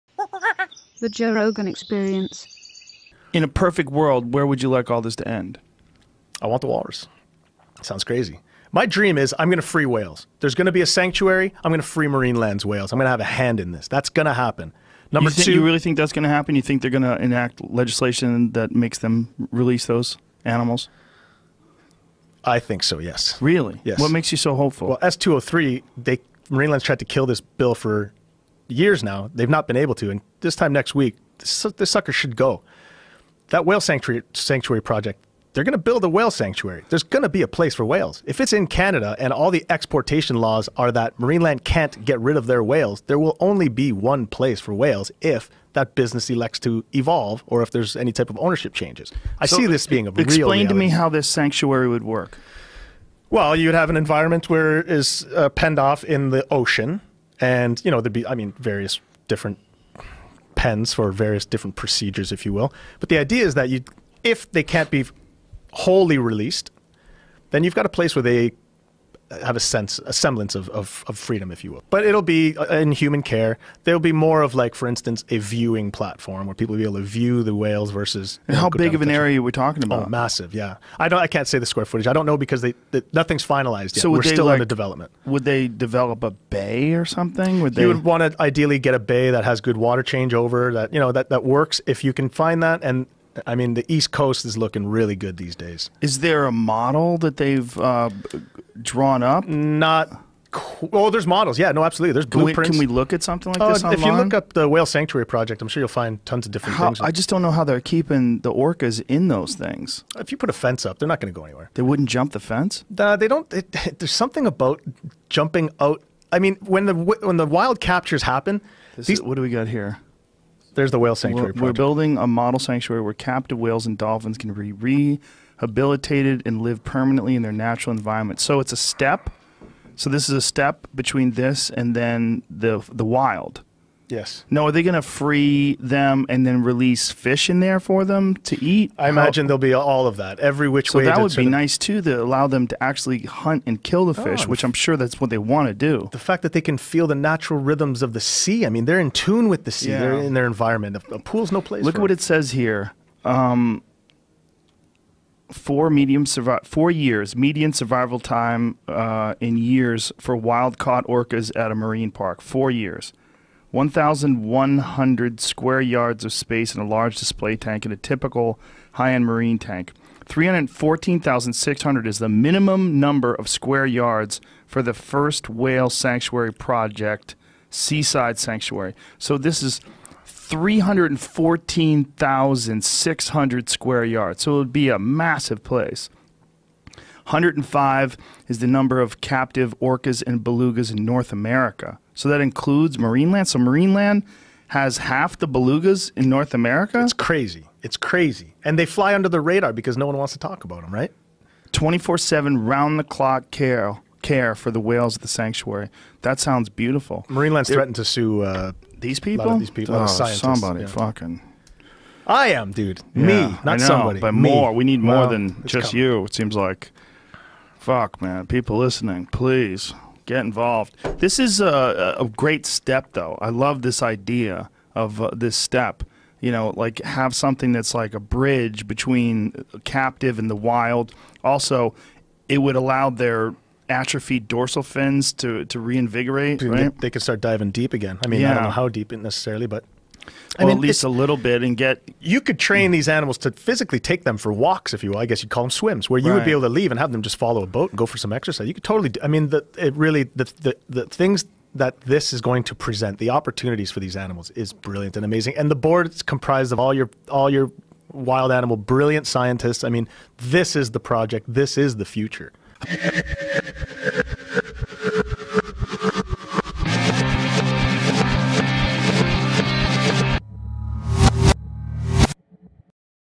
Joe Rogan & Phil Demers "My Dream is to Free Whales"
Phil Demers tells Joe Rogan about his dream to free whales.